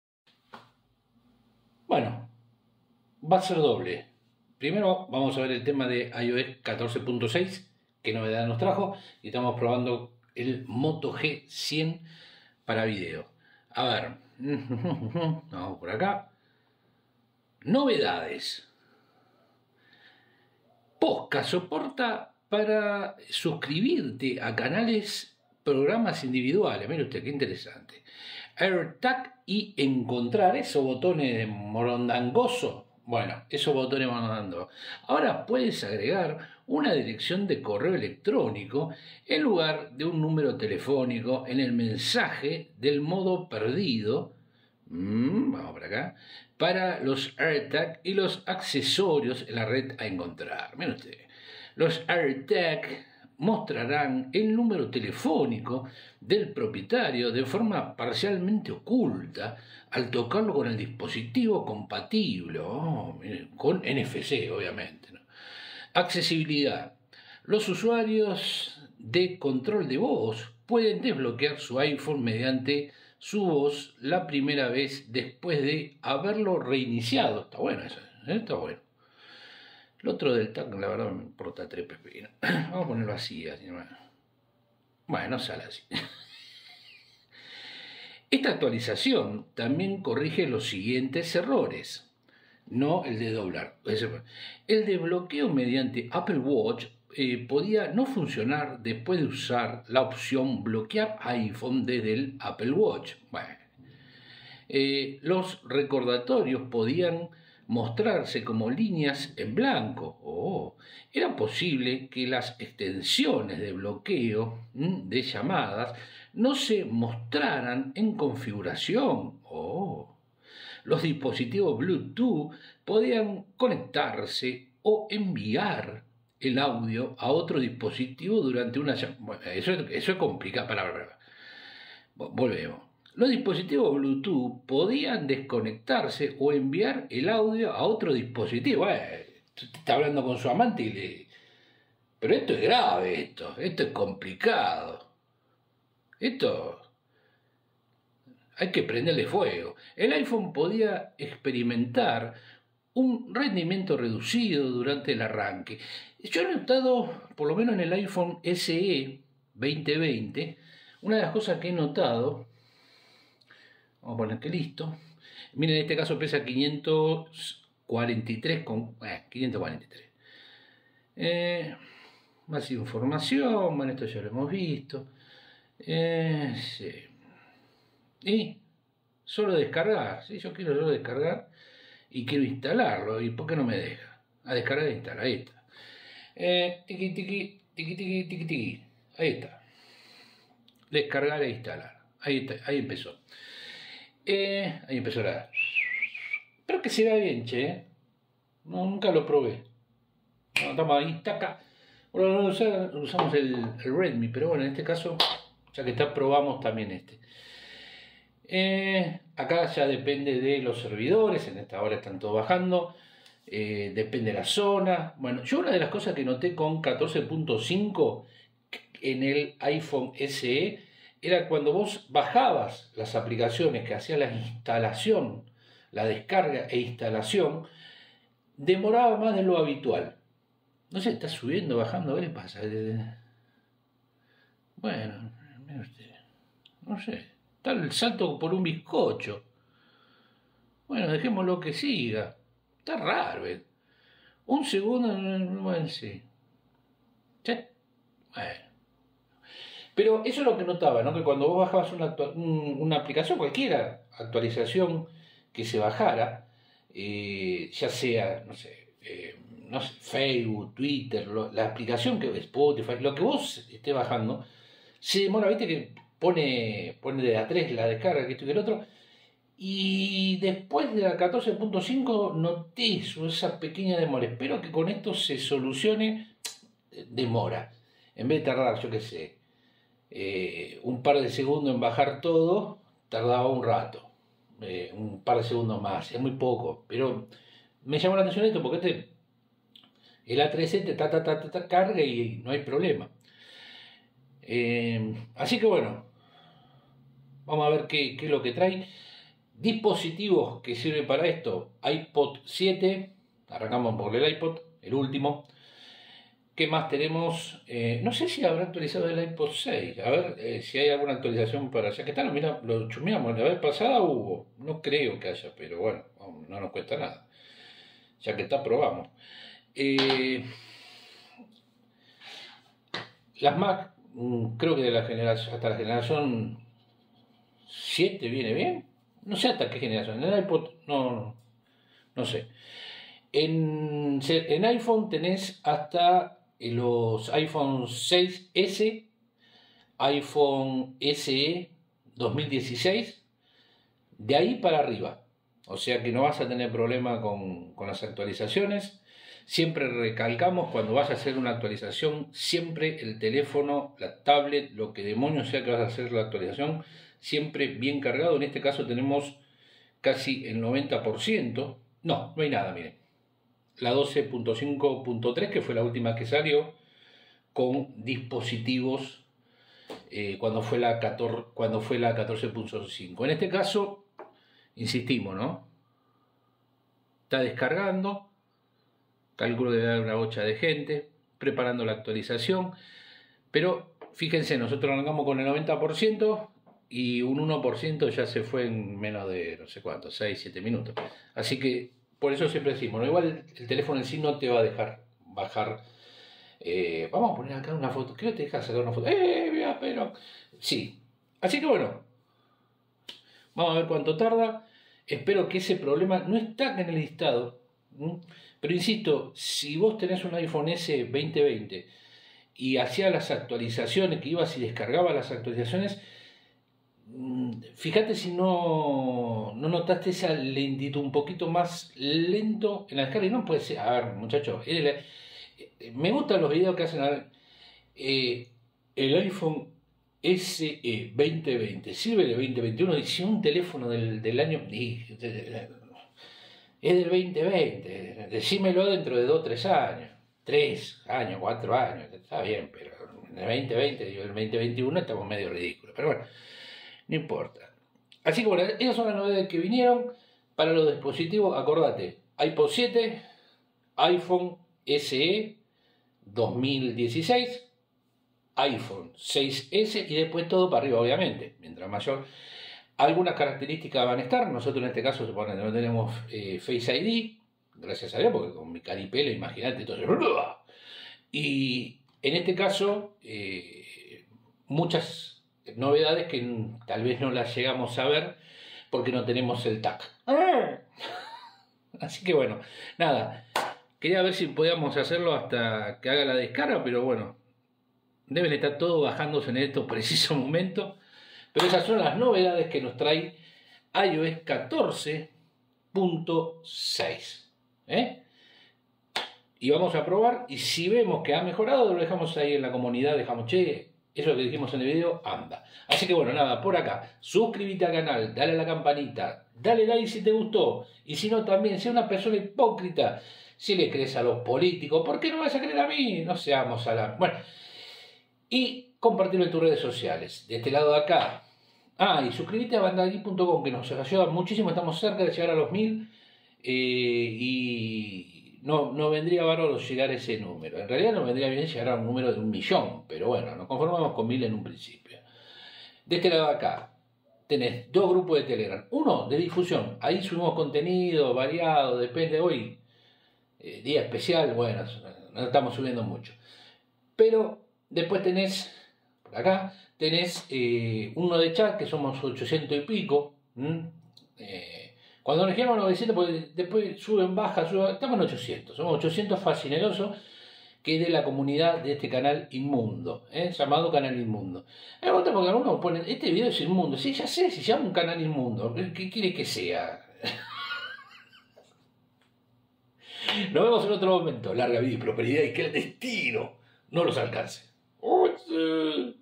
IOS 14.6 Novedades
Llego una nueva actualización , veamos que trae
Bajo su responsabilidad